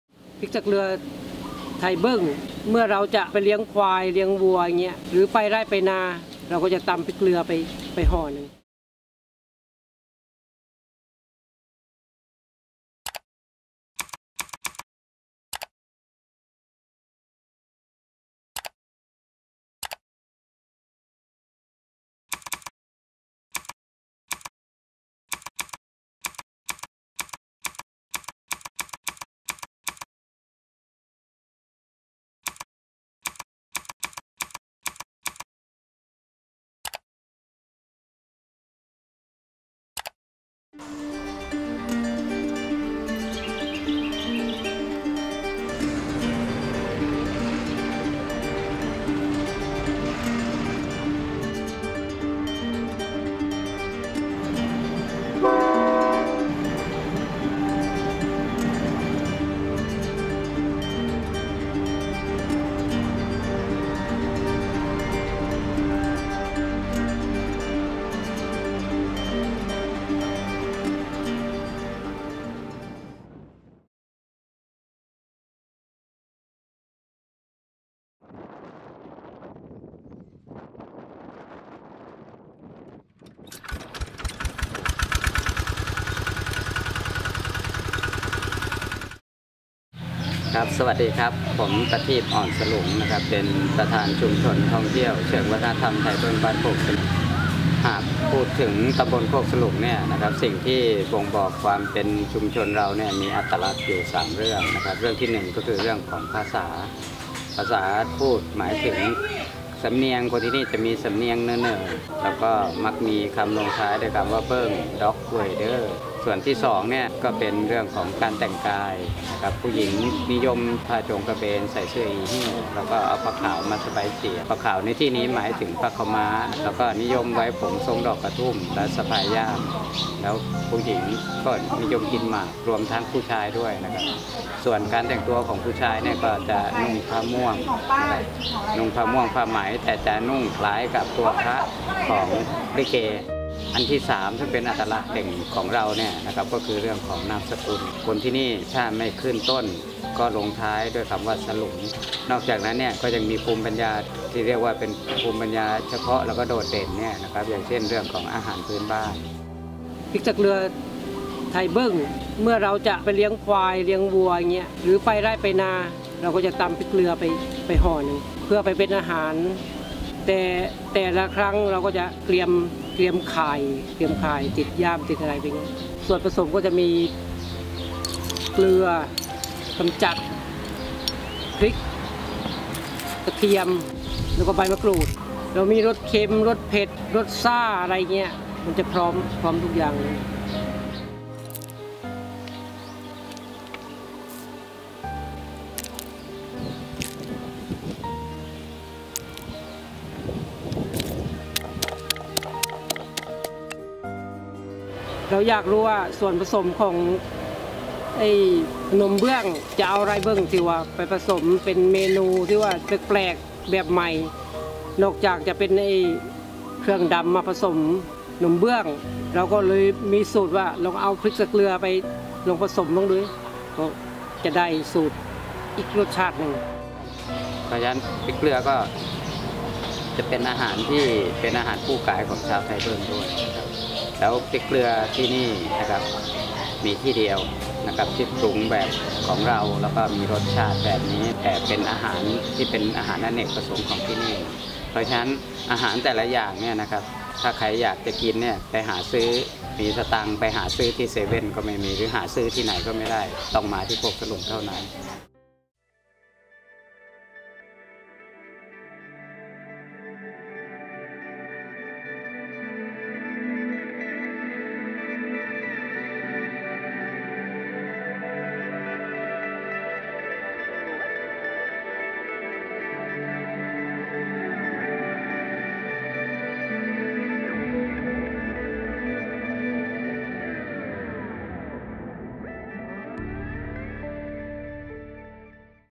NAVANURAK by Victory | สืบสานภูมิปัญญาพริกตะเกลือ
ชื่อผลงาน : สืบสานภูมิปัญญาพริกตะเกลือ
ชื่อทีม : Victory
สถาบัน : มทร.ธัญบุรี
โจทย์ที่ได้รับ : พริกตะเกลือ

การแข่งขัน NAVANURAK Story Creator Challenge 2020 ประชันไอเดียเล่าเรื่องผ่านสื่อดิจิทัล เพิ่มมูลค่าของข้อมูลทางวัฒนธรรมให้ตอบโจทย์ทางด้าน ท่องเที่ยว การศึกษา การอนุรักษ์ และไลฟ์สไตล์ ย้อนรอยสัมผัส "ความอบอุ่นแบบดั้งเดิม" ที่บ้านไทยเบิ้ง จ.ลพบุรี ด้วยการเดินทางด้วยรถไฟลอยนํ้าเรียนรู้วิถีชีวิตของชาวบ้านยุคเก่าที่ยังคงรักษาวัฒนธรรม ขนบธรรมเนียม ที่ส่งต่อกันมารุ่นต่อรุ่น